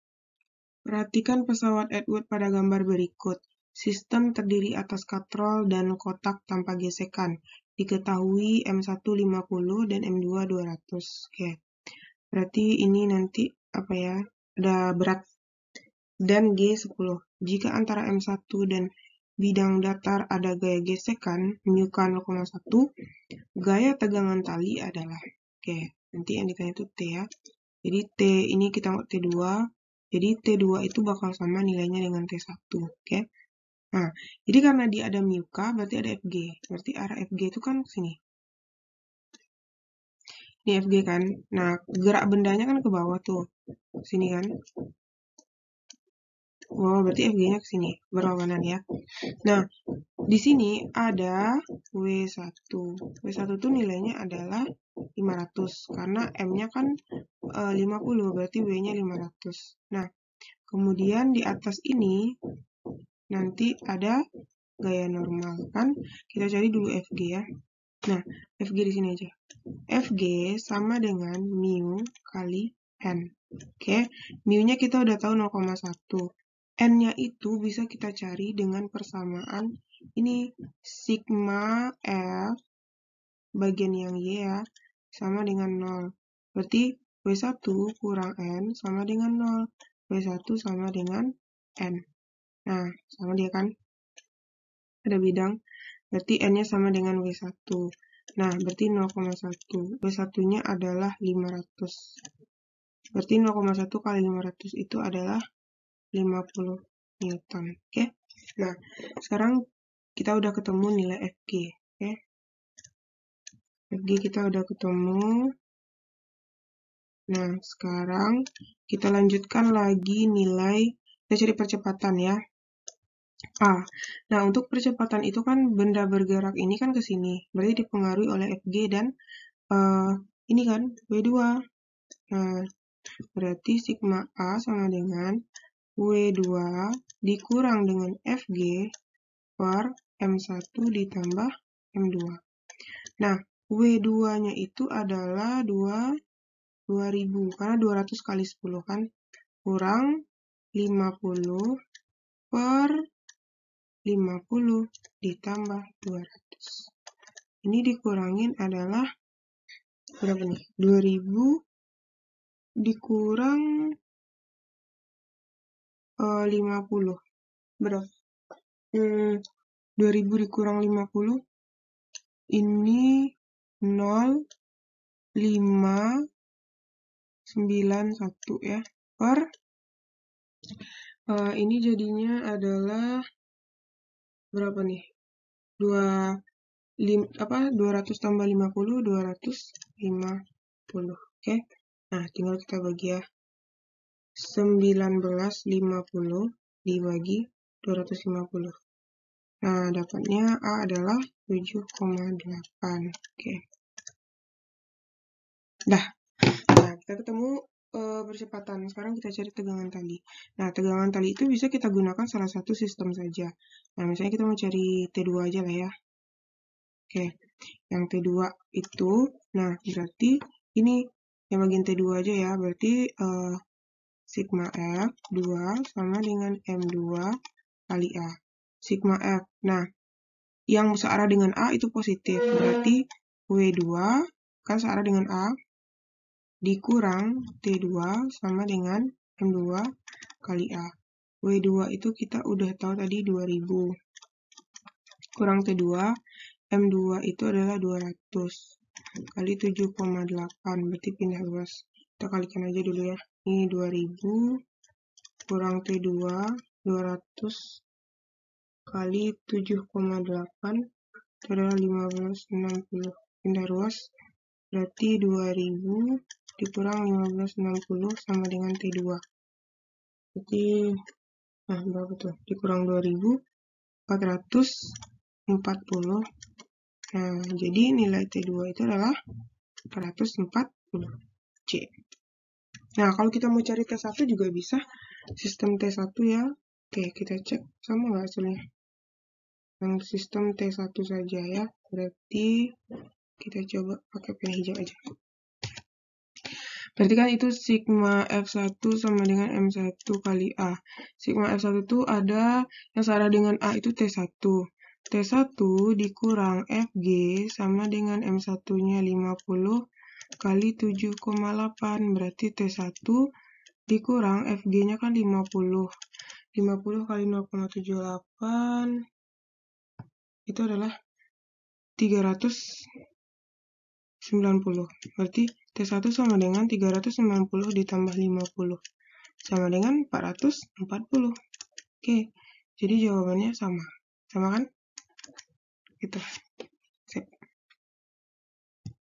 PH DINAMIKA GERAK no 15 fisika intanpariwara belajar fisika
perhatikan pesawat adwood pada gambar berikut!
sistem terdiri dari atas katrol dan kotak tanpa gesekan. diketahui m1 50 kg , m2 200 kg dan gravitasi 10 m/s^2 jika antara mi dan bidang datar ada gaya gesek dengan koefisien gesek kinetis adalah 0,1 ( massa katrol diabaikan), gaya tegangan tali sebsar.